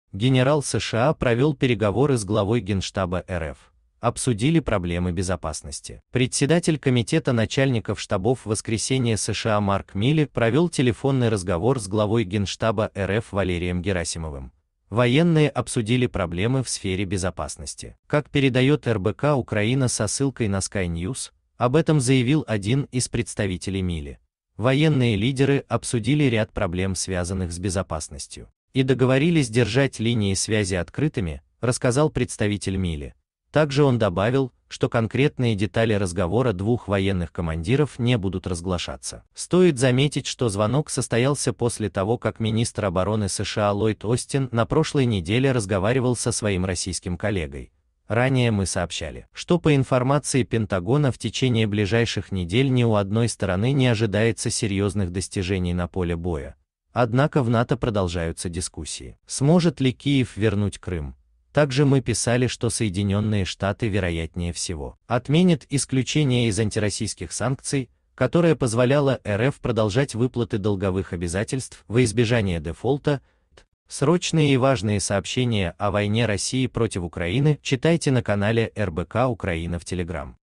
Генерал США провел переговоры с главой Генштаба РФ: обсудили проблемы безопасности.
РоссийскаяФедерация СоединенныеШтатыАмерики ГенштабРФ ВалерийГерасимов Председатель Комитета начальников штабов ВС США Марк Милли провел телефонный разговор с главой Генштаба РФ Валерием Герасимовым. Военные обсудили проблемы в сфере безопасности. Как передает РБК-Украина со ссылкой на SkyNews, об этом заявил один из представителей Милли. "Военные лидеры обсудили ряд проблем, связанных с безопасностью, и договорились держать линии связи открытыми", - рассказал представитель Милли. Также он добавил, что конкретные детали разговора двух военных командиров не будут разгл